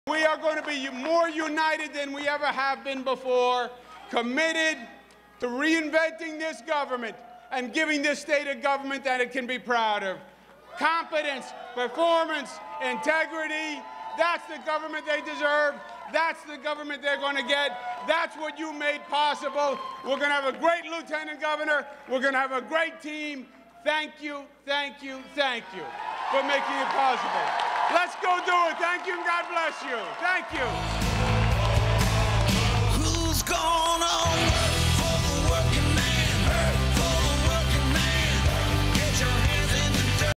Andrew Cuomo Elected NYS Governor
The newly-elected Andrew Cuomo thanks his supporters in New York City after beating Republican Carl Paladino.